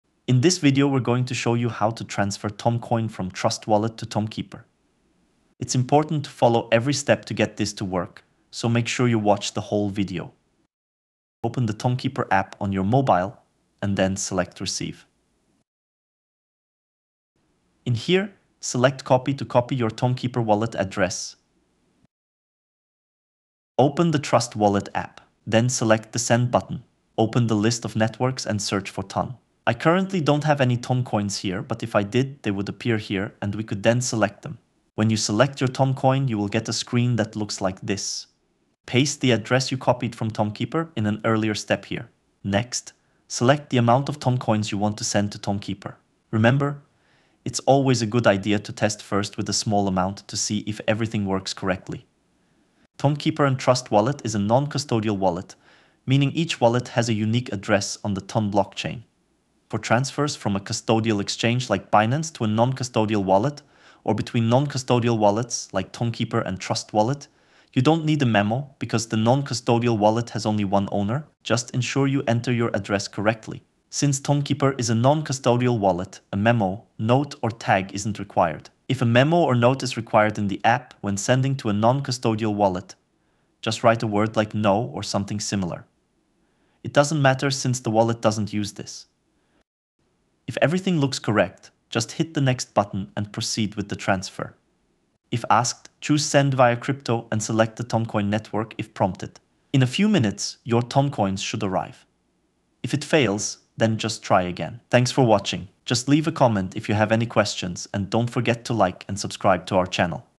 How to Transfer Toncoin from Trust Wallet to Tonkeeper | How To Send Ton From Trust W. to Tonkeeper
How to Transfer Toncoin from Trust Wallet to Tonkeeper | How To Send Ton From Trust Wallet to Tonkeeper. Easy step-by-step-tutorial.

In this video we will show you how to send Toncoin ($TON) from Tonkeeper Wallet to Trust Wallet with a quick and easy step-by-step-tutorial.